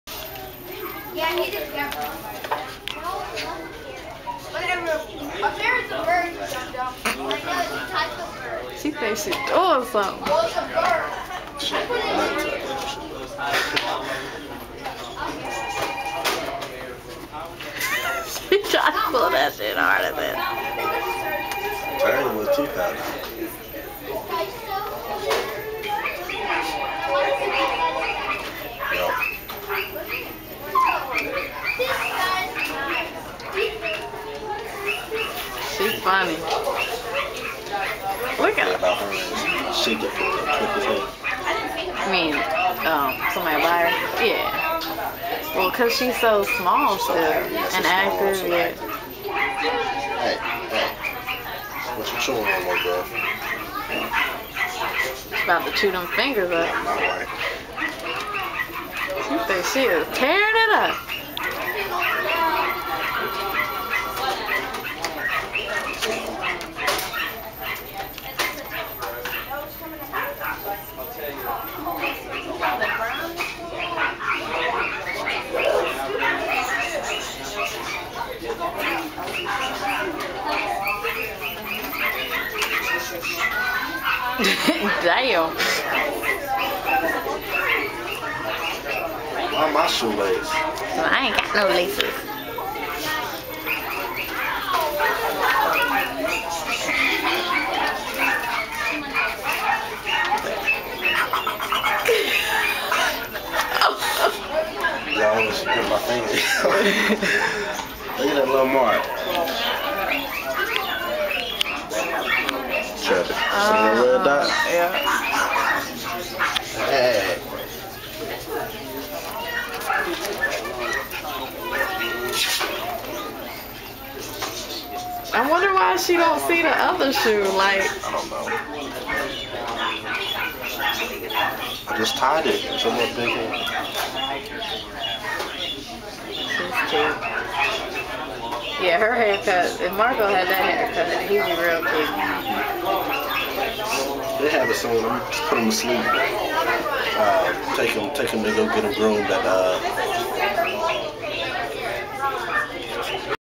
Puppy eats shoelace
Cute mini snauchzer at pet store